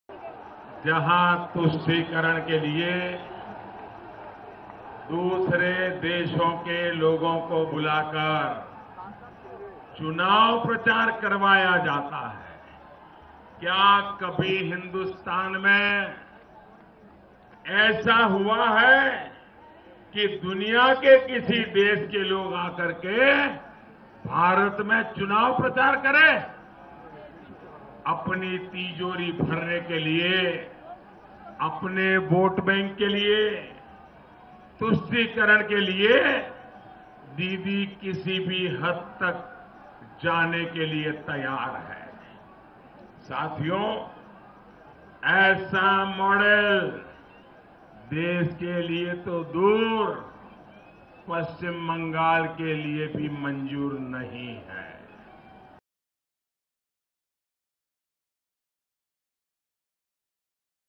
'She can to go any extent for votes': PM Modi attacks Mamata Banerjee
Prime Minister Narendra Modi on Saturday accused West Bengal Chief Minister Mamata Banerjee of going to any extent for votes. The Prime Minister was speaking at a rally in West Bengal’s Buniadpur. PM Modi's rally in Bengal came three days ahead of the third phase polling of Lok Sabha election 2019. The Bharatiya Janata Party (BJP) has set a target of winning 23 of 42 Lok Sabha seats in West Bengal.